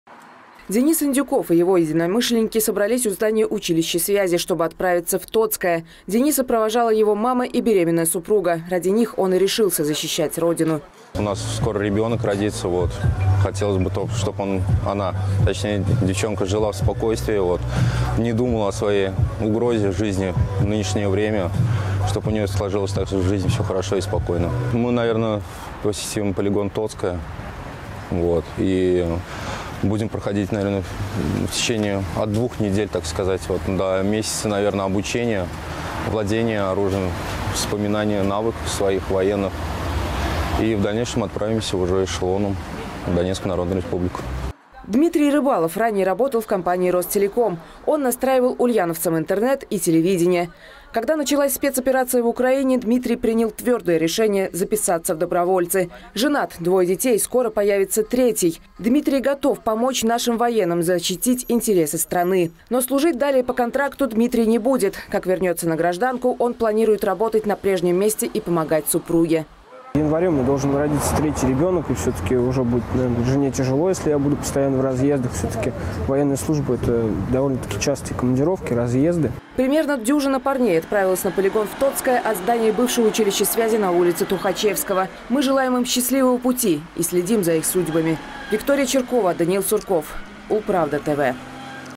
Ради семьи и Отчизны. Ульяновские добровольцы отправились защищать Родину
Парни из Ульяновска поехали на военный полигон в Тоцкое, чтобы пройти курс молодого бойца и отправиться в Донецкую Народную Республику. Ранним утром мужчины собрались в межвидовом учебном центре на Тухачевского. Ради чего ульяновцы едут в командировку на самом деле – узнаете в нашем материале.